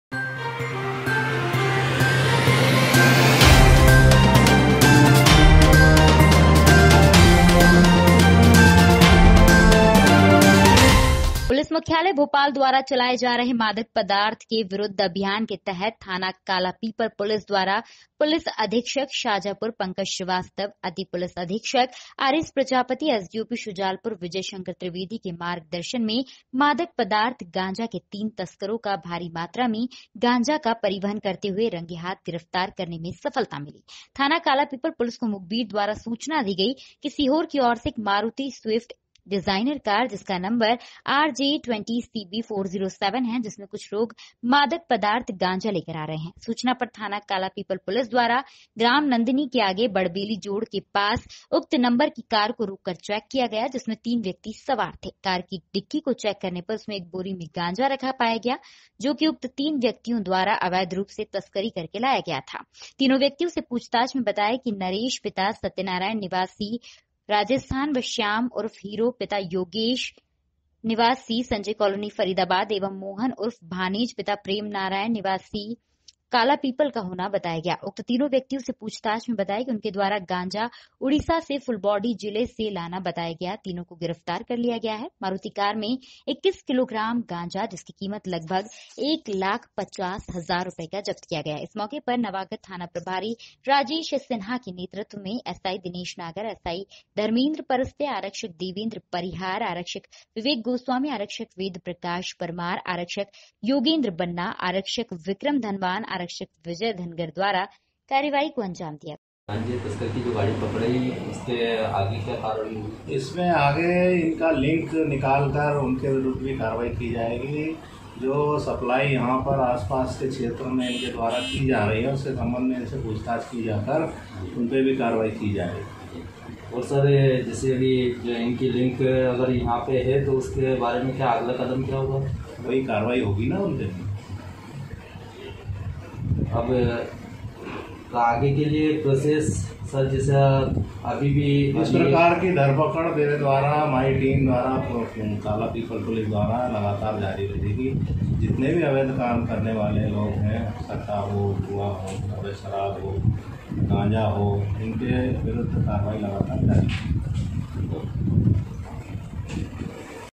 शाजापुर जिले के थाना कालापीपल मे नव गत थानाप्रभारी की टीम द्वारा 21 किलो मादक पदार्थ गांजा
पुलिस मुख्यालय भोपाल द्वारा चलाए जा रहे मादक पदार्थ के विरुद्ध अभियान के तहत थाना कालापीपल पुलिस द्वारा पुलिस अधीक्षक शाजापुर पंकज श्रीवास्तव अति पुलिस अधीक्षक आर एस प्रजापति एसडीओपी सुजालपुर विजय शंकर त्रिवेदी के मार्गदर्शन में मादक पदार्थ गांजा के तीन तस्करों का भारी मात्रा में गांजा का परिवहन करते हुए रंगे हाथ गिरफ्तार करने में सफलता मिली थाना कालापीपल पुलिस को मुखबिर द्वारा सूचना दी गई की सीहोर की ओर से एक मारुति शिफ्ट डिजाइनर कार जिसका नंबर आर जे 20 सी बी 4071 है जिसमें कुछ लोग मादक पदार्थ गांजा लेकर आ रहे हे सूचना पर थाना कालापीपल पुलिस द्वारा ग्राम नंदिनी के आगे बड़बेली जोड़ के पास उक्त नंबर की कार को रोककर चेक किया कार में 3 व्यक्ति सवार थे कार की डिग्गी को चेक करने पर उसमें एक बोरी में गांजा रखा पाया गया जोकि उक्त तीन व्यक्तियों द्वारा अवैध रूप से तस्करी करके लाया गया था तीनों व्यक्ति से पूछताछ में बताया कि नरेश पिता सत्यनारायण मीणा निवासी राईतल जिला बूंदी राजस्थान व श्याम उर्फ हीरो पिता योगेश यादव निवासी सेक्टर 23 संजय कॉलोनी फरीदाबाद एवं मोहन उर्फ भानेज पिता प्रेम नारायण पुष्पा कर माली निवासी चमन मोहल्ला खोकरा कला थाना कालापीपल का होना बताया उक्त तीनों व्यक्ति से पूछताछ में बताया कि उनके द्वारा गांजा उड़ीसा से फुलबॉडी जिले से लाना बताया गया तीनों को गिरफ्तार कर लिया गया है मारुति कार मैं 21 किलोग्राम गांजा  जिसकी कीमत लगभग एक लाख पचास का जप्त किया गया इस मौके पर  नवगत थाना प्रभारी राजेश सिन्हा के नेतृत्व में एसआई दिनेश नागर एसआई धर्मेंद्र परस्ते आरक्षक देवेंद्र परिहार आरक्षक विवेक गोस्वामी आरक्षक वेद प्रकाश परमार आरक्षक योगेंद्र बन्ना आरक्षक विक्रम धनवान आरक्षक विजय धनगर द्वारा कार्रवाई को अंजाम दिया गया